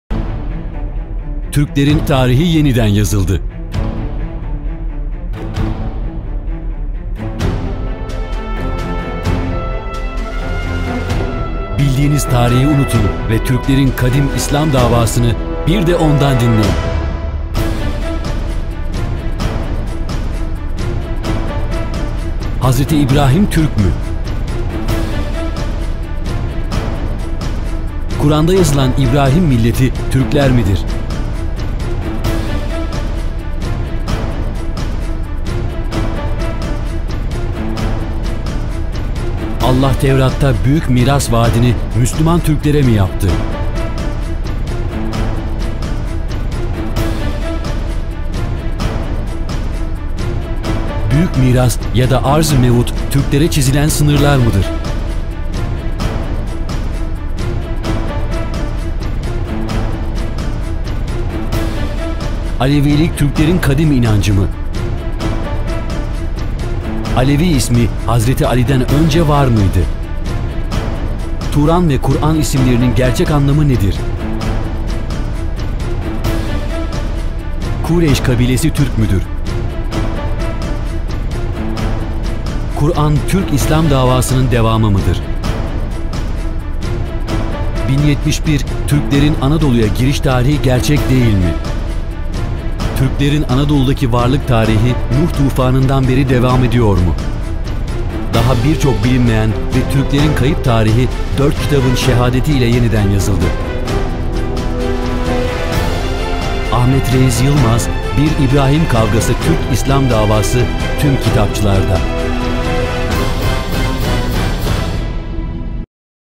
Bir İbrahim Kavgası TÜRK-İSLAM DAVASI
Ahmet Reyiz YILMAZ 'ın Kaleminden Bir İbrahim Kavgası TÜRK-İSLAM DAVASI adlı türklerin kayıp tarihine ışık tutan muhteşem eseri.